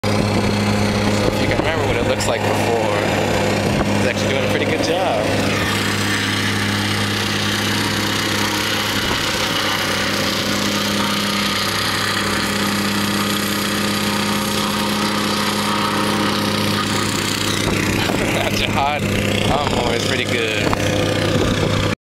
TCN lawnmower
Me just filming a TCN using a ground flatner.